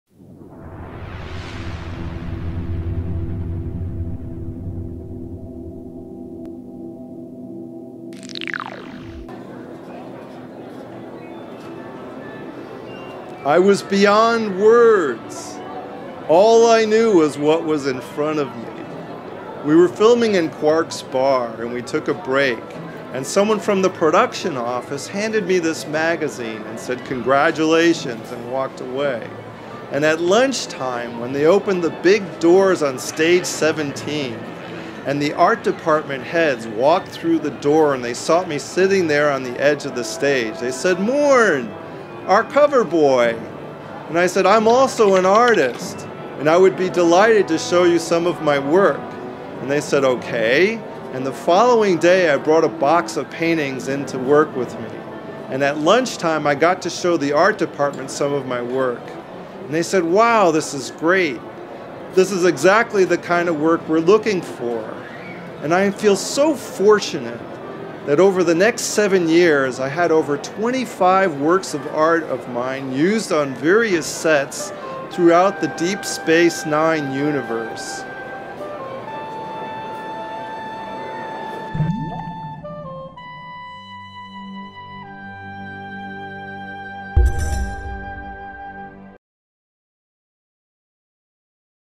The Art Of Mark
MORN SPEAKS is a web series by Mark Allen Shepherd who played  "Morn" on Star Trek: Deep Space Nine for 7 seasons without uttering a single word on screen. Mark recalls what it was like to play Morn on Star Trek: Deep Space Nine and it´s significance in context with the Star Trek universe.